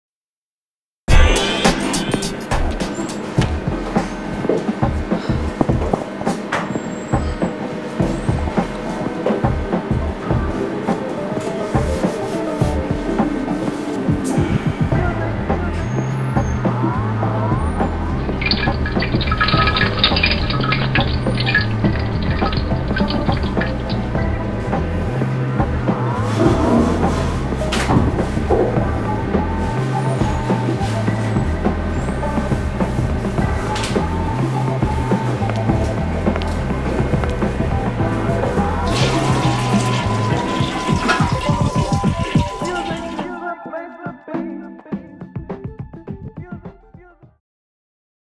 ToiletThink - Lounge
Noi donne quando usiamo un bagno pubblico abbiamo sempre paura delle malattie.
E quando facciamo sesso? Riflettici su toiletthink.it